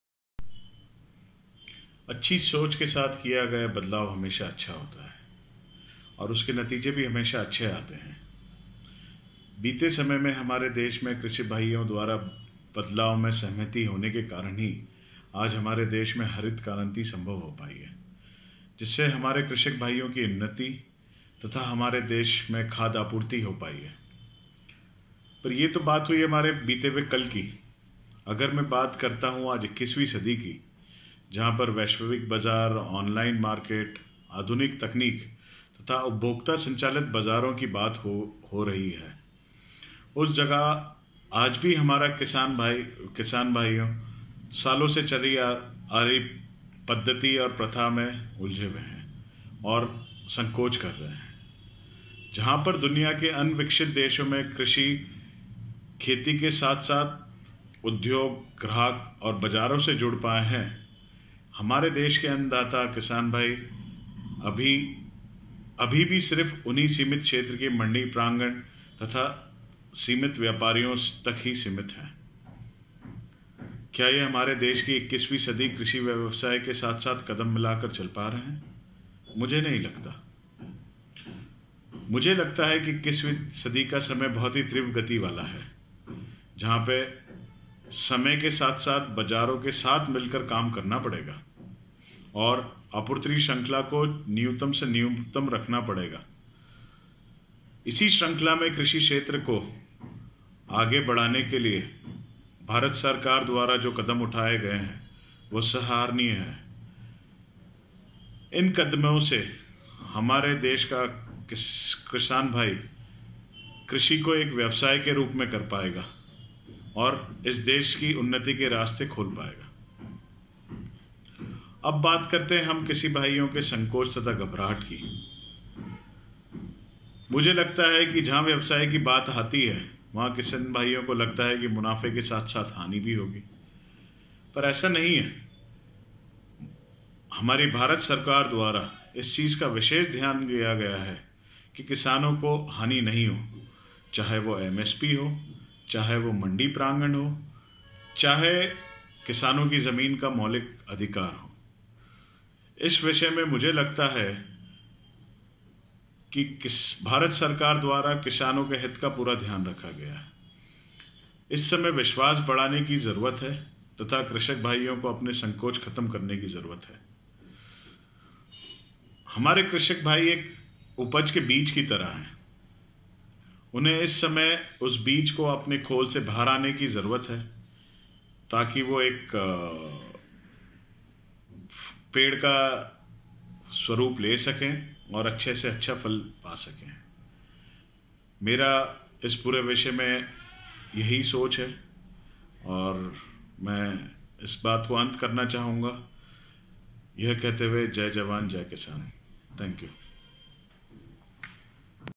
मिनिमम सपोर्ट प्राइस नहीं मैक्सिमम प्राइस का ज़माना है , CII ने भी कृषि सुधार बिल का किया समर्थन ।
मिनिमम सपोर्ट प्राइस नहीं मैक्सिमम प्राइस का ज़माना है-- कृषि सुधार बिल एक अच्छा कदम  ,
कन्फेडरशन ऑफ़ इंडियन इंडस्ट्री - CII ने भी कृषि सुधार बिल का किया समर्थन।

मिनिमम सपोर्ट प्राइस (MSP) नहीं मैक्सिमम प्राइस का ज़माना है- इनसाइट टीवी से खास बातचीत कन्फेडरशन ऑफ़ इंडियन इंडस्ट्री के ग्रामीण और शहरी संयोजक पंकज गोयल से भी बात की ।  पंकज गोयल ग्रामीण क्षेत्र के औद्योगिक परिवेश से आते है और इस विषय पर उनका किसानो के साथ सीधा संपर्क है इसलिए उनका इस बारे में विचार महत्वपूर्ण हो जाता है।

पंकज गोयल ने बताया की अच्छी सोच के साथ किया गया बदलाव हमेशा अच्छा होता है और उसके नतीजे भी हमेशा अच्छे आते है।  पिछले दशकों में किसान भाइयों की सहमति के कारण ही देश में हरित क्रांति संभव हो पाई है।  इसकी वजह से ही हमारे कृषक भाइयो की उन्नति संभव हुई और देश  खाद्यन्नों के मामले में आत्म निर्भर हो पाया है। यदि वर्तमान की बात की जाए तो आज वैश्विक बाजार में ऑनलाइन बाजार और आधुनिक तकनीकी की बात हो रही है ऐसी परिस्थिति में हमारे कृषक भाई आज भी एक सीमित और पारम्परिक बाजार व्यवस्था में कैद है।